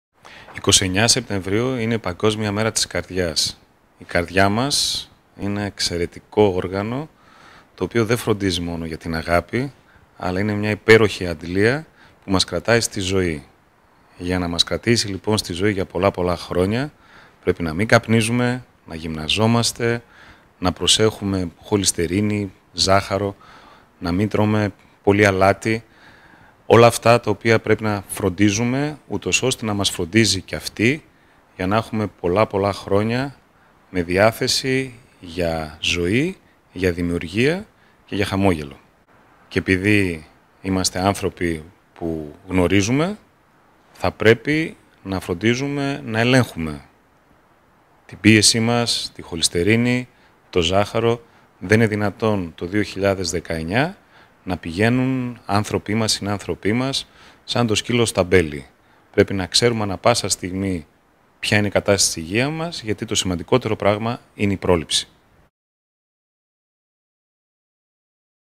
Ο Κωνσταντίνος Ζέρβας για την Παγκόσμια Ημέρα Καρδιάς
Καλή διατροφή, άθληση και προληπτικούς ελέγχους προτείνει στο μήνυμα του, ο Δήμαρχος Θεσσαλονίκης,  Κωνσταντίνος Ζέρβας, για την Παγκόσμια Ημέρα Καρδιάς, που γιορτάζεται στις 29 Σεπτεμβρίου.